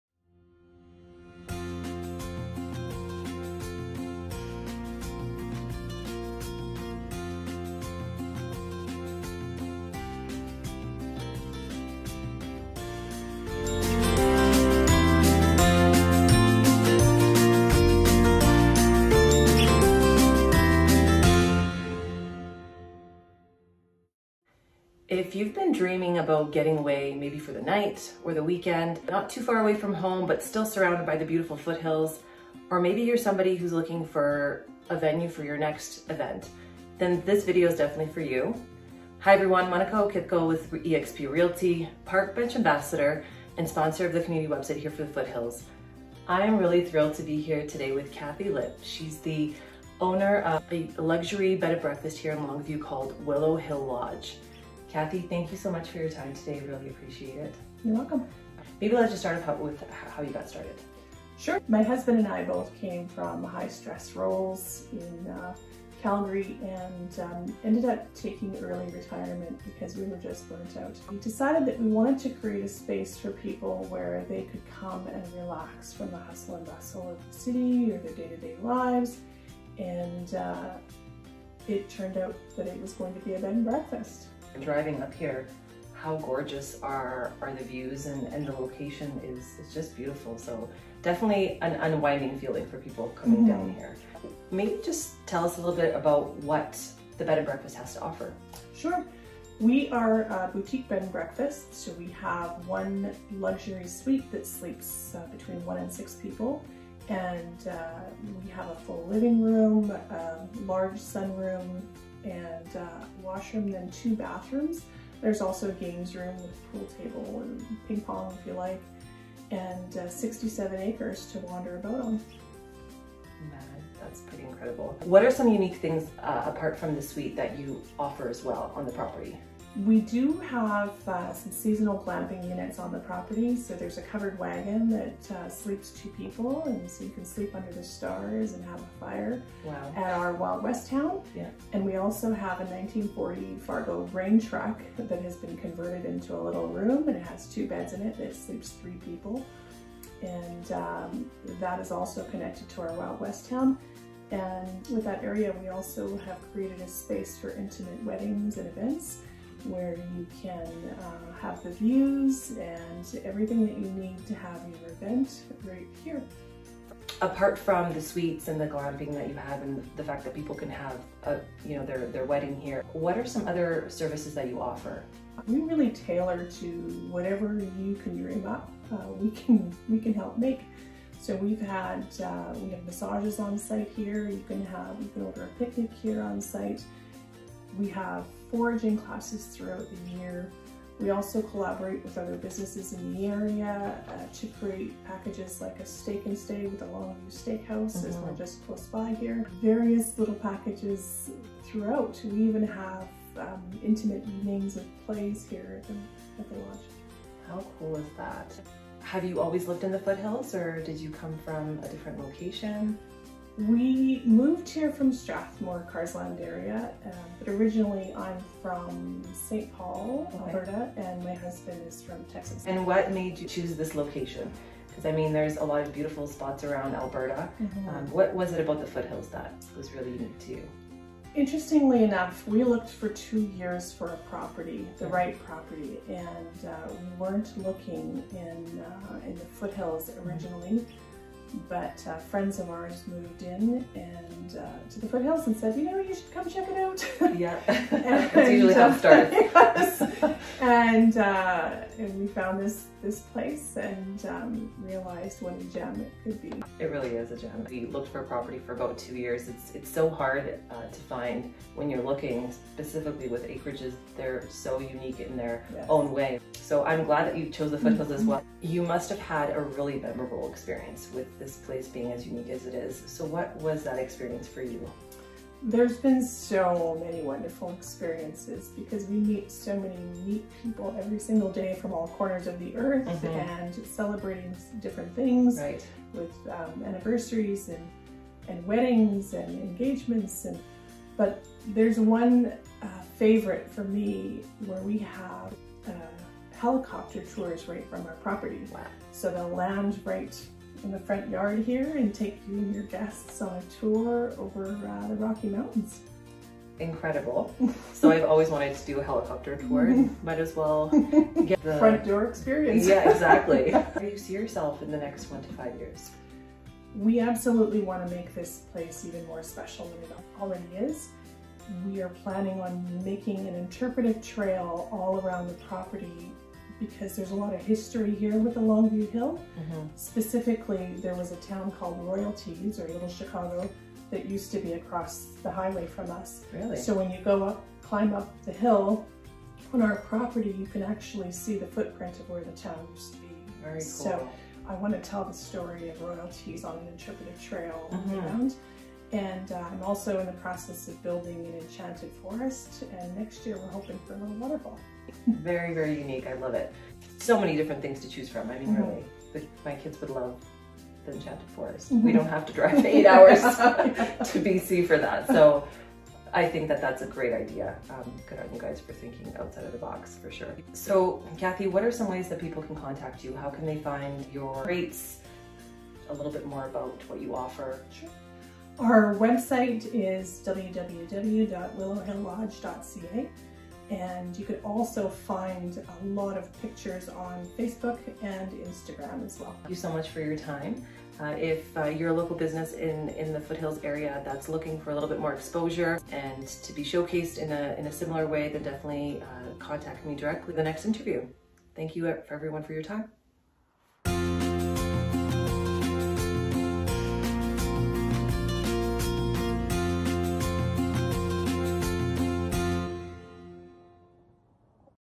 Foothills Community Interview - Willow Hill Lodge - Longview, AB
✨In this video I got the opportunity to interview Cathy Lipe, the  owner of a luxury Bed and Breakfadg in the Longview, Willow Hill Lodge..

Willow Hill Lodge was a dream for many years before coming to reality in 2020.
James and Cathy are the Experience Curators here at Willow Hill.
They purchased the property near Longview, Alberta in 2018 and began to lovingly restore and renovate it so they could share the beautiful surroundings with friends and family. Their goal is to be able to build a place for people to come and stay in a peaceful setting where they can forget about the world outside, even if only for a short while. The sound of nature and quiet is all around you at Willow Hill Lodge.

The home they offer for the Bed and Breakfast is a log home built on a hillside, and boasts some of the best views of the valley and the Alberta Rocky Mountains.

The Bed and Breakfast is in a separate suite in the home, and offers 2 private bedrooms that share common areas. The whole suite can also be booked as one unit, in which case, there would be complete privacy. Booking the entire suite gives you the best experience at Willow Hill Lodge.

FB: @ Willow Hill Lodge

✨ I Sponsor the Local Foothills Community website to get to know the amazing businesses in the area and  be able to help them grow their visibility and brand awareness in this magical place!